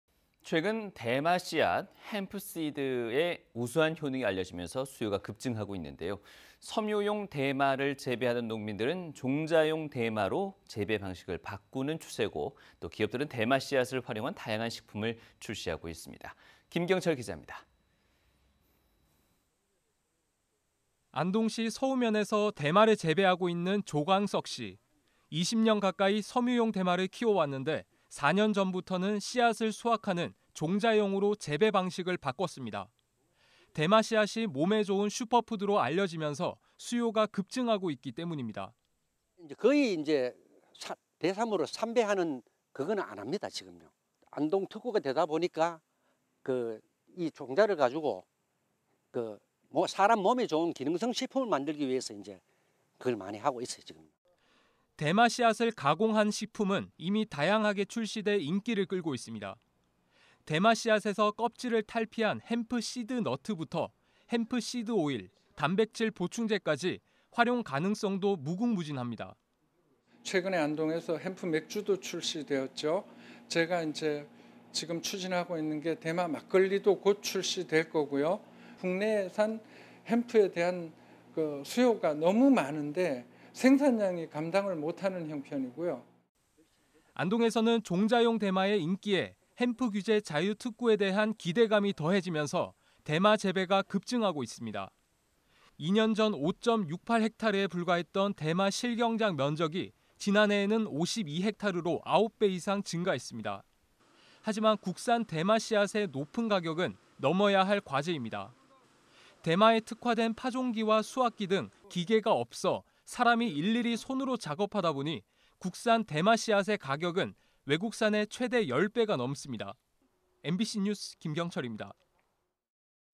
대마 씨앗이 슈퍼푸드?..수요 급증 | 대구MBC뉴스
최근 대마 씨앗, 즉 헴프 씨드의  우수한 효능이 알려지면서, 수요가 급증하고 있는데요.

섬유용 대마를 재배하던 농민들은  종자용 대마로 재배 방식을 바꾸는 추세고, 기업들은 대마 씨앗을 활 용한 다양한 식품을 출시하고 있습니다.

내용: 대마 씨앗이 슈퍼푸드?..수요 급증 - 김경철 기자 (안동M)